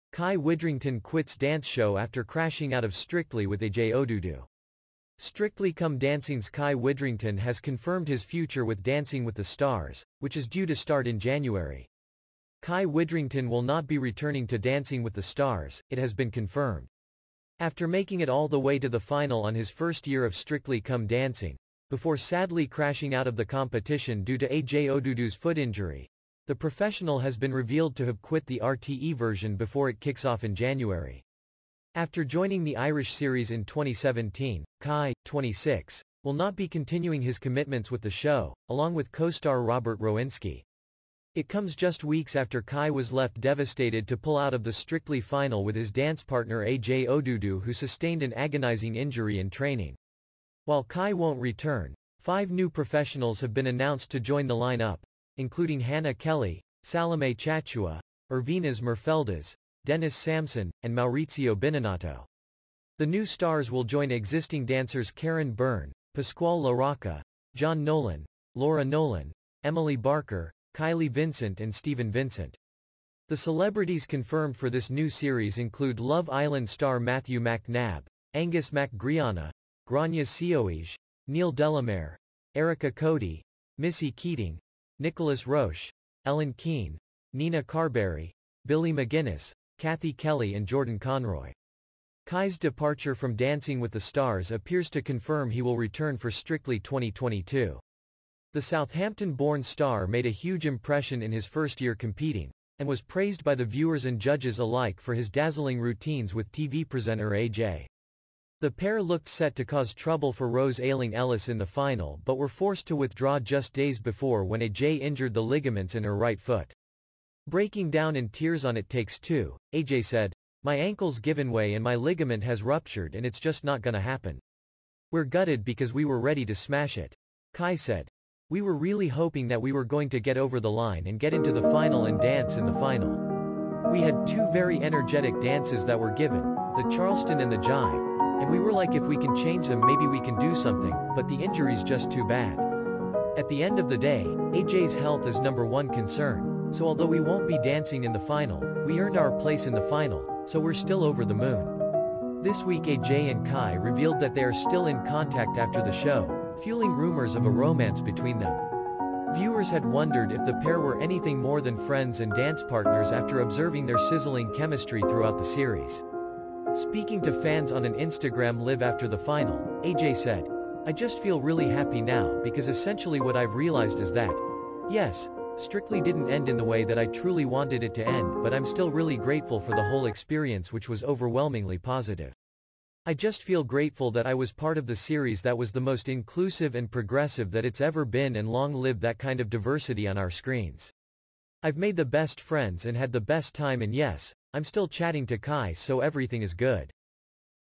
Kai Widdrington quits dance show after crashing out of Strictly with AJ Odudu
Strictly Come Dancing's Kai Widdrington has confirmed his future with Dancing with the Stars, which is due to start in January.